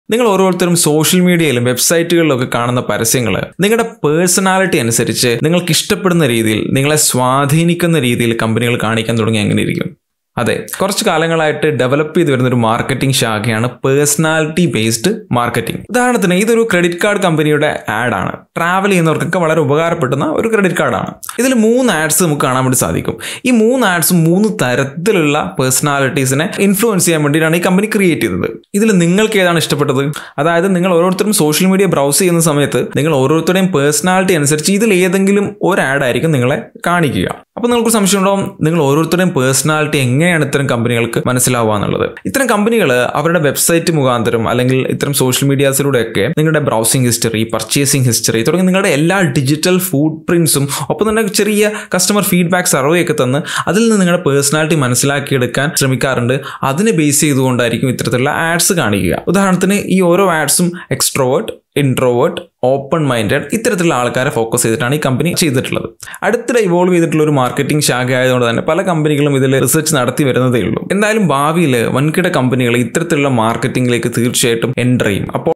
Personality Based Marketing talk by Jeevan Uthaman
Marketing Tip Malayalam about Personality-Based Marketing